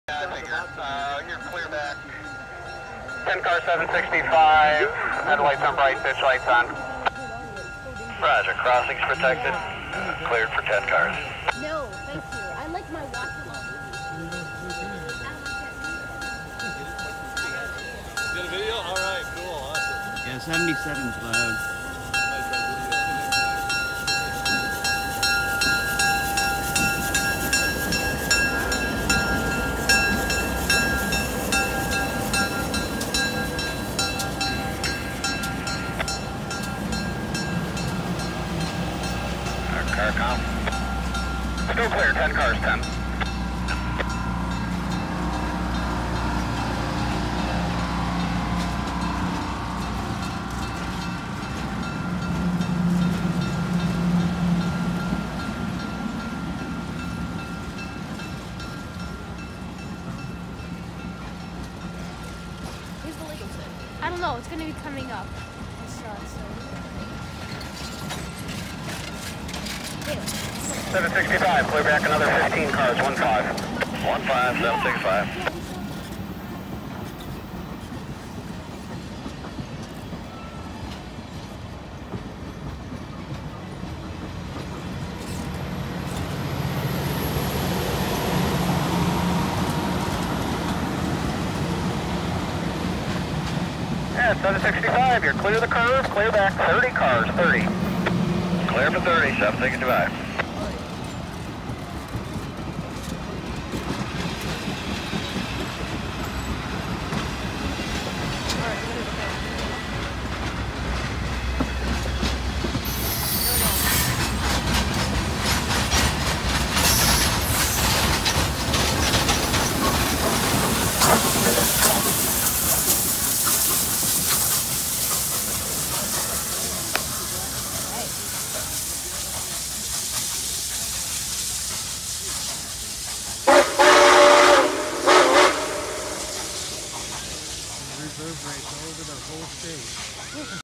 CVSR 6777 Backing Up NKP 765 For Second Runby
after the first runby, 6777 pulls the train north around the curve so 765  can do a second runby